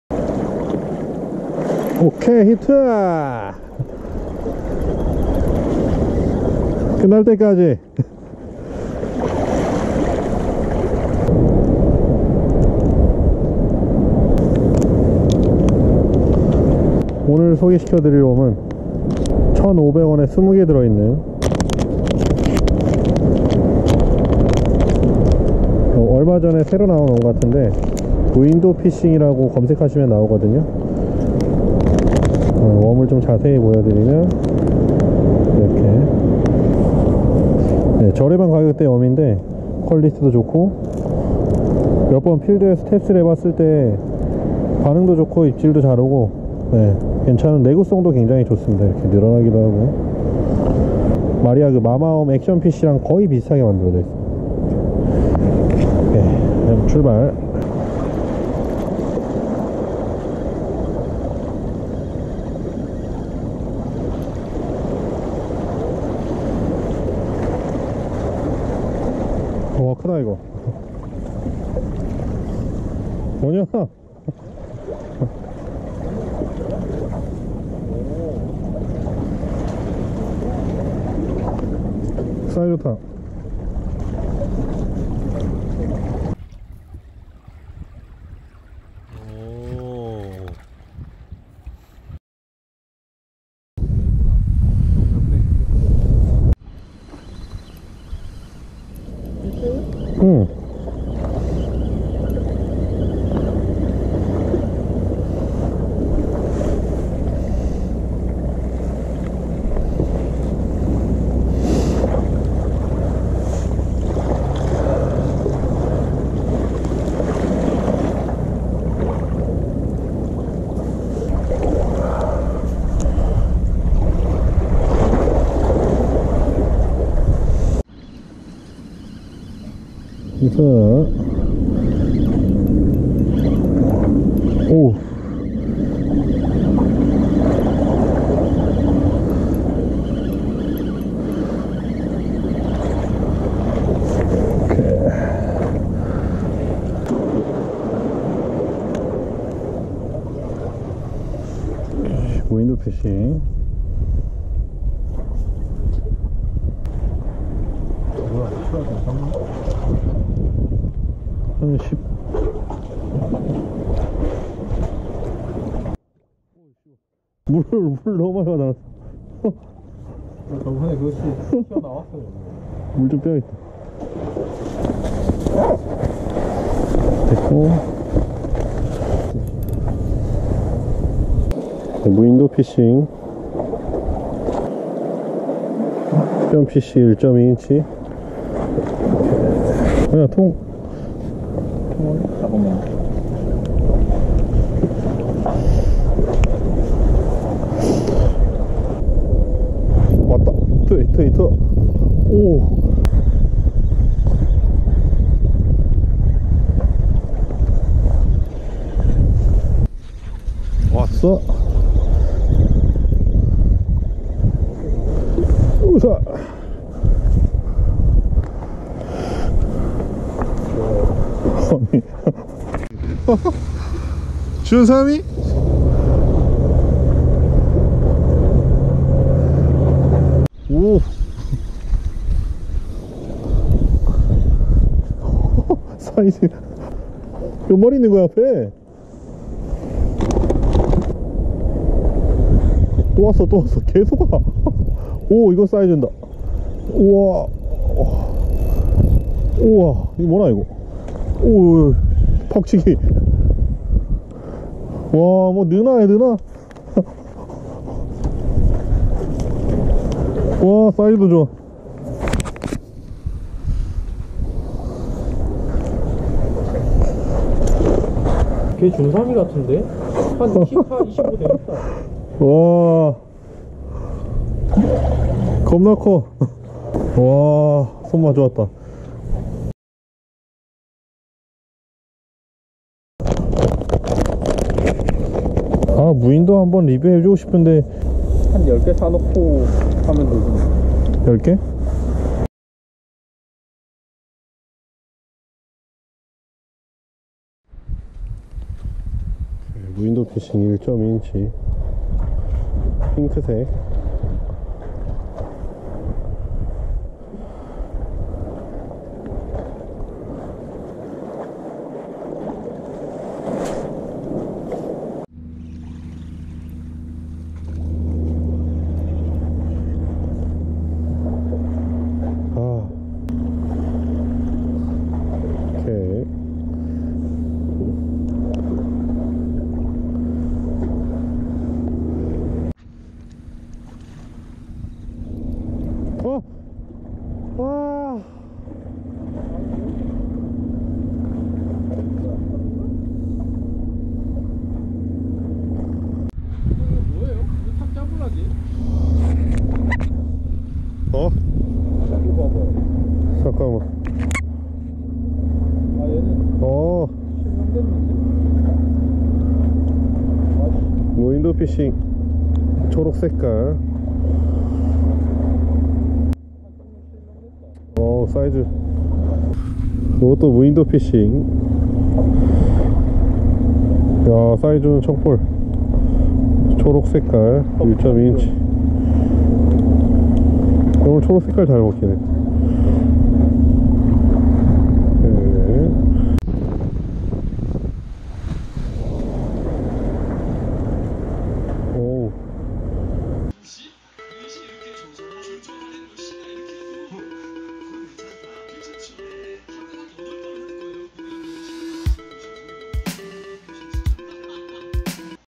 볼락 웜 추천 가성비 퀄리티 대박 워킹 루어낚시 필수 액션피쉬 rock fish
안녕하세요 비룡입니다~
직접 돈주고 샀으며 홍보나 광고는 1도없는
솔직하게 리뷰 추천 영상입니다!!
영상에서는 전부 리뷰웜으로 다잡은영상입니다~

얼마전에 새롭게사용하게된 웜이 가격 성능 내구성이 좋아서 10회이상 출조하면서 테스트해보고 충분히 볼락에게 잘먹힌다는 확신이생겨 여러분들에게 웜을 추천합니다~!